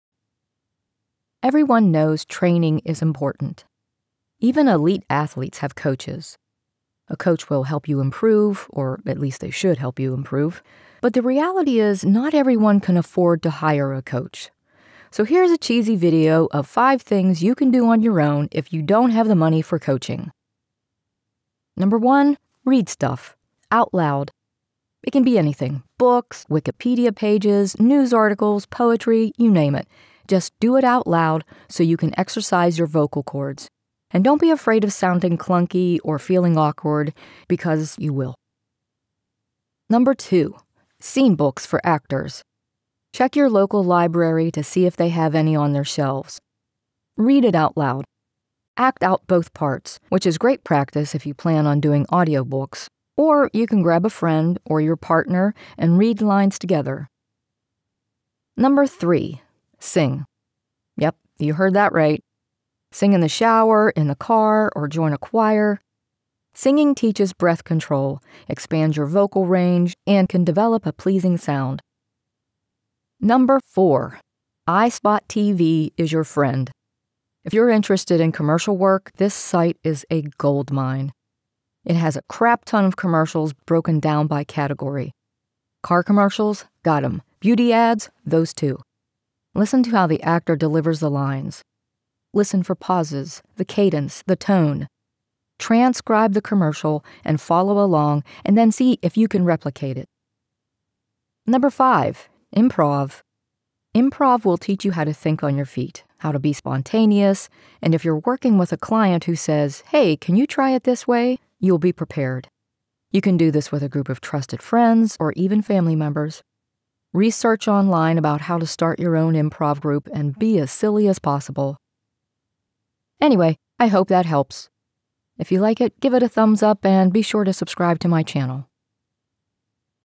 5 Ways Voice Actors Can Train on Their Own
Not every new voice actor can afford to hire a coach or get training. So, what do you do until you can? Here are five tips to fill in the blanks while saving your pennies to hire a coach and/or start a training program.